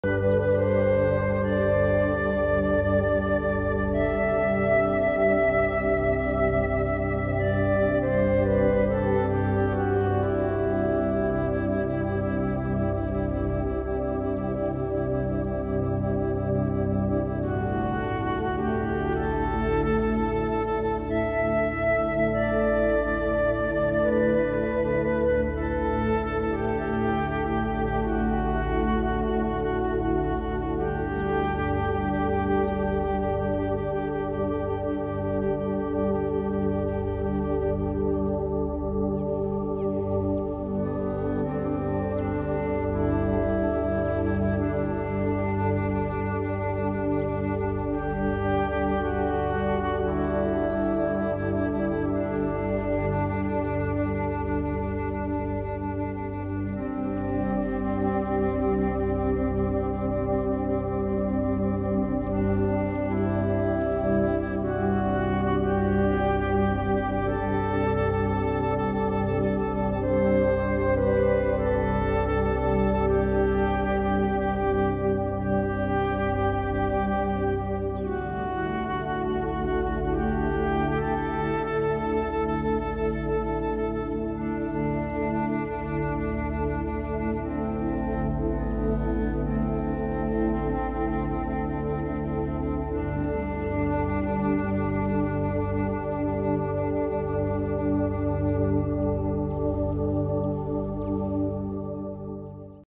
meditation in 10 minutes you can feel the worl meaning
this is short and unexpected meditation video that can you to relax and feel sutsfuction